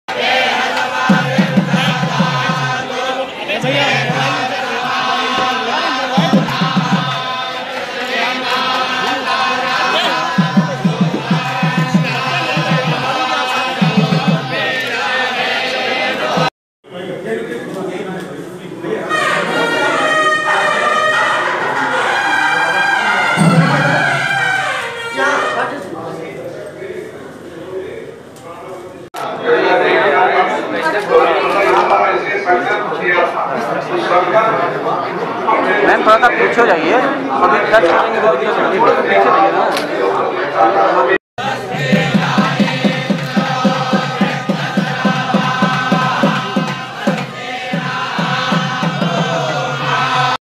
प्रयागराज के शंकर विमान मंडपम मंदिर में योगी आदित्यनाथ ने की पूजा
प्रयागराज: योगी आदित्यनाथ एक दिवसीय दौरे पर धार्मिक आयोजन के बुलावे पर प्रयागराज पहुंचे. आगमन के दौरान उन्होंने संगम तट पर स्थिति श्री आदि शंकर मंदिर में चल रहे चार दिवसीय कुंभाभिषेक कार्यक्रम में पूजन अर्चन की.